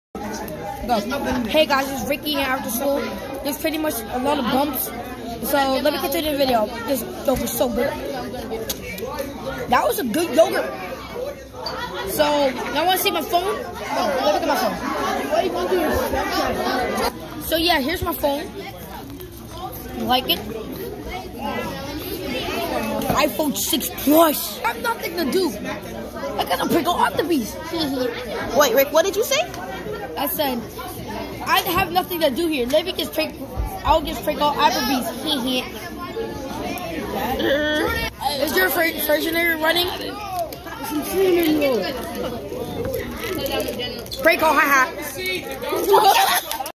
After school vlog(prank call)
brook we prank called apple bees